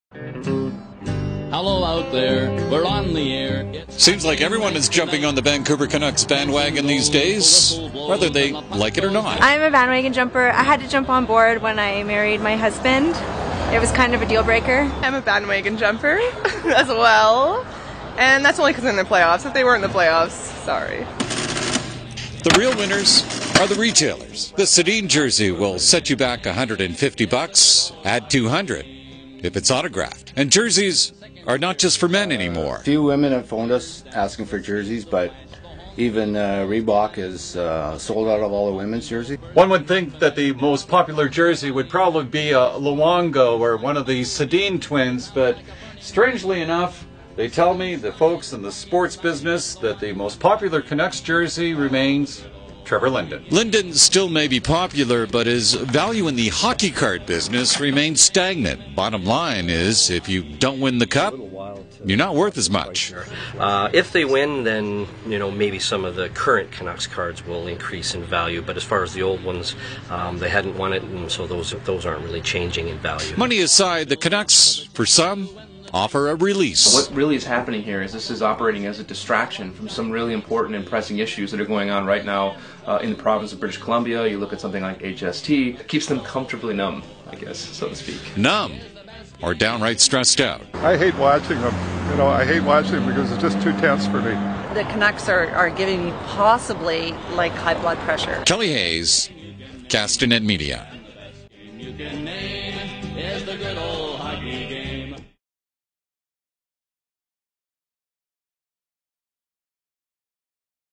Canucks fever contagious
Jumping on the Canucks bandwagon can be hazardous to your health and your wallet. Castanet's Kelly Hayes reports.